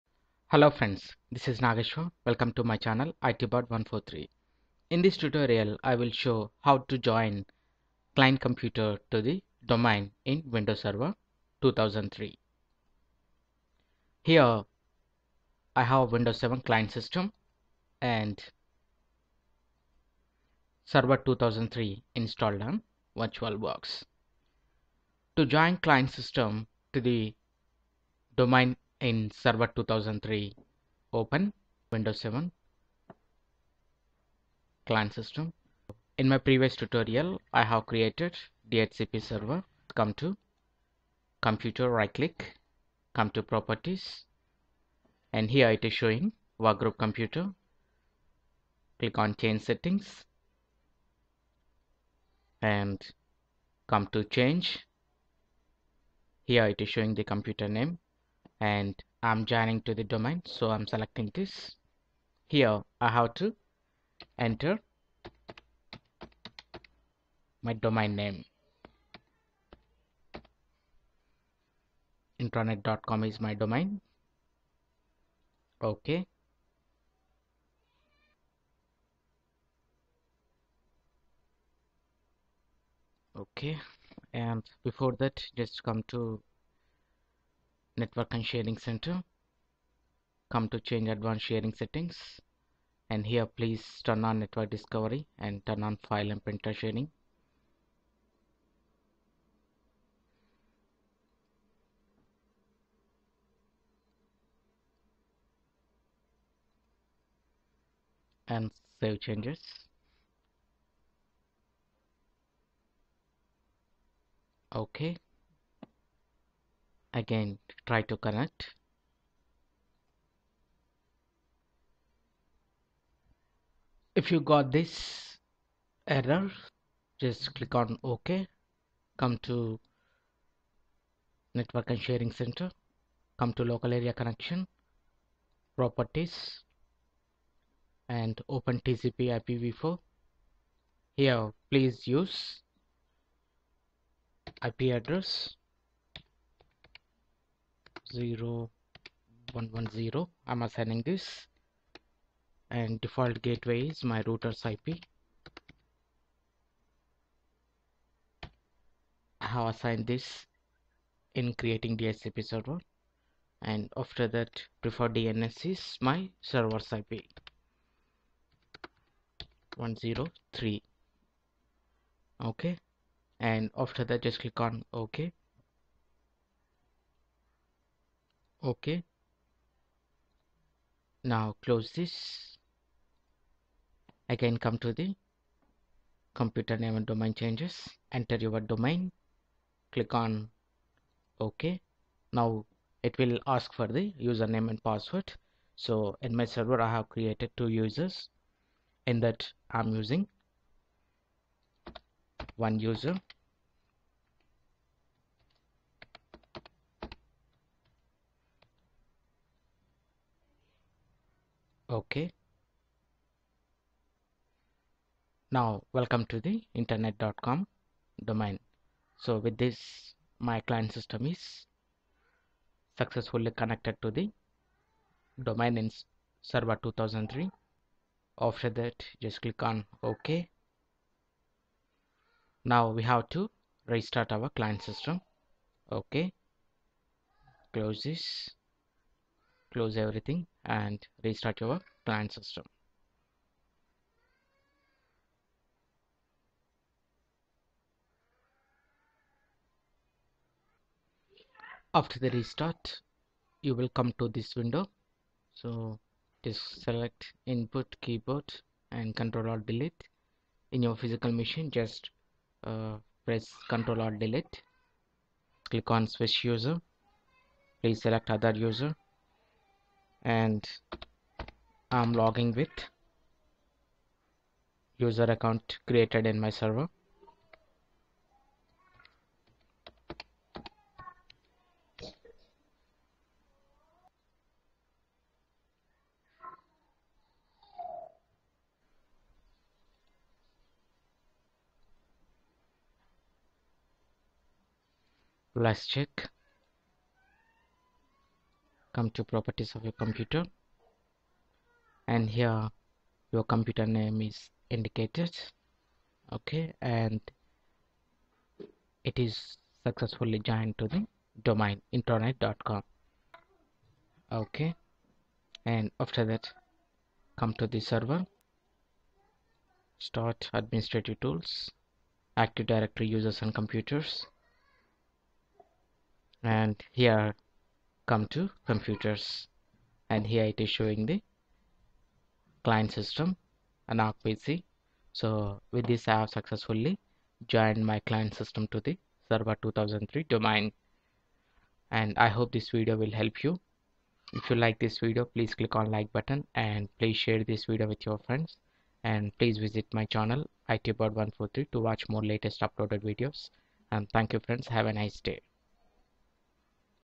Server 2003 - How to join Client systems to Domain in Windows Server 2003
In this tutorial, I have shown joining of client systems to domain in Windows server 2003 in step by step process.